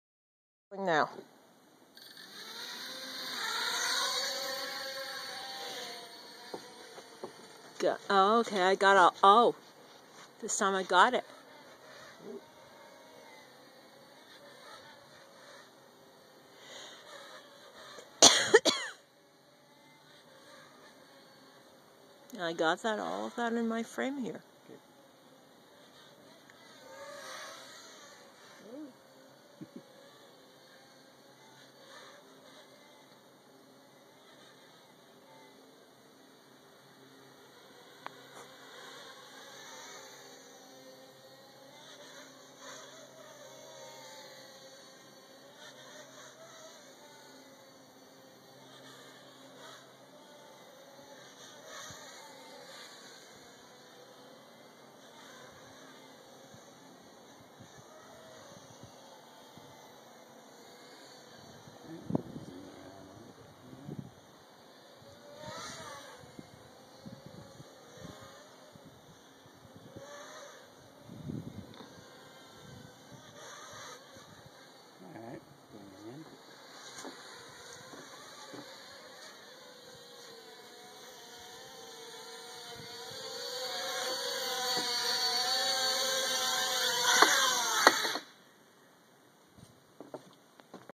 HeliMax 1SQ in the Backyard
Flying my HeliMax 1SQ in the backyard, doing some quick-flips and acrobatic flying.  There was a bit of wind, but the little quad flies right through it.  Thanks to my Lex for shooting the video.